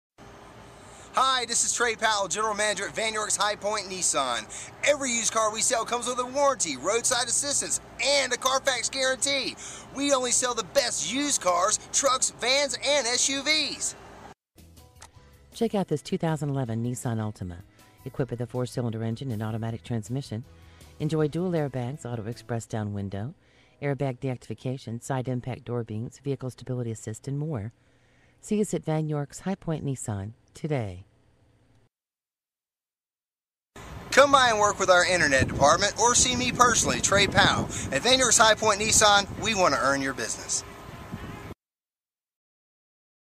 Used 2011 Nissan Altima 2.5 S Greensboro, Winston Salem High
Used 2011 Nissan Altima 2.5 S Greensboro, Winston Salem High Point, Kernersville NC Vann York's, imagine driving this Super Black 2011 Nissan Altima, equipped with a 4 Cyl. engine and an automatic transmission with only 18,142 miles. enjoy an exceptional 32 miles to the gallon on this great car with features like Cruise Control, Power Steering, Power Door Locks, Tachometer, Side Air Bag System, Dual Air Bags, Auto Express Down Window, Traction Control System, Multi-Function Steering Wheel, and much more. Enjoy the drive and have peace of mind in this 2011 Nissan Altima. see us at Vann York's High Point Nissan today!

miles- 18,142

vin- 1N4AL2AP1BC168613

1810 S. Main ST.